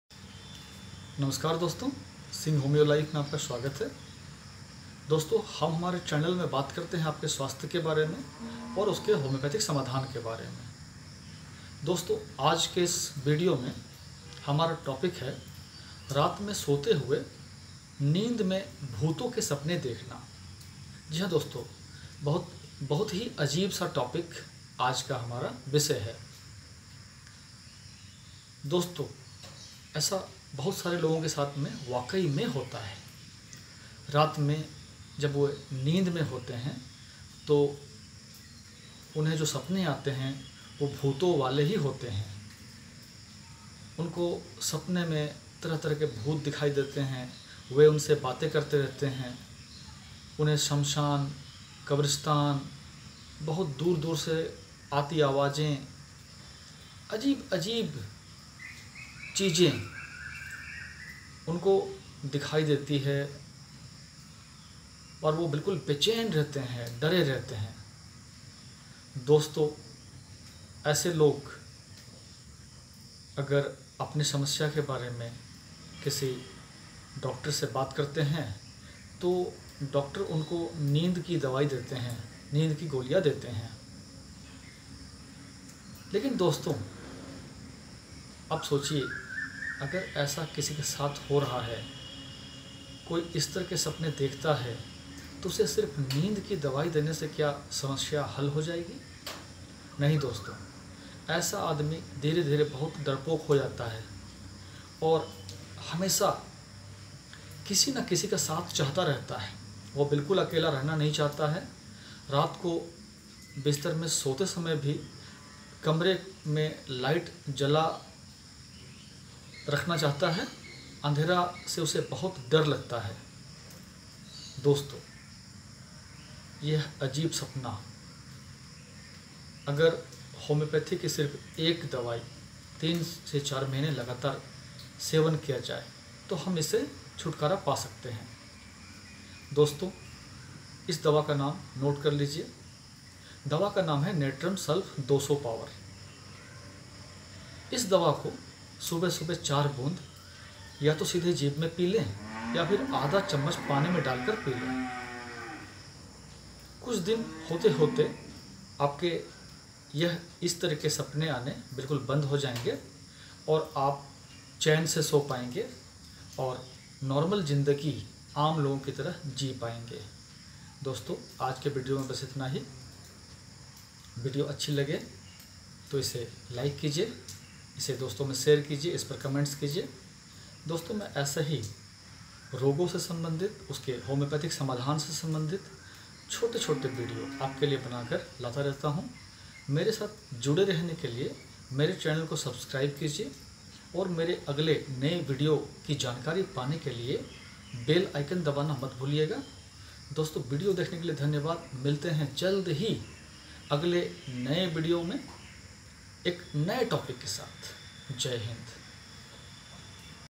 BHOOTON KE SAPNE/डरावने सपने आना/NIGHTMARE/बुरे सपने/BHOOTON KE SAPNE AANA KA HOMIOPATHIC ILAAJ
KUCH LOGON KO HAR RAAT NIND MEIN BHOOTON KE DARAVANE SAPNE AATE HAI. UNHE SAPNE MEIN AJIB AJIB SI AAVAJE SUNAI DETI HAI. VE SAPNE MEIN BHOOTON SE BATE KARTE RAHTE HAI. UNHE ANDHERE MEIN MEIN KAFI DAR LAGTA HAI. VE AKELA NAHIN RAHNA CHAHTE HAI. HAMESA KISI KA SATH CHAHTE HAI. DOCTOR KO DIKHANE PER VE SIRF NIND KI DAWA DETE HAI JISKE KARAN ROGI HAMESA SUST RAHTA HAI. HOMIOPATHY KI SIRF EK DAWA NATRUM SULPH 200 SUBAH KHALI PET 4 BUND LAGATAR 3 MONTH SEVAN KARNE SE YAH ROG BILKUL THIK HO JAYEGA.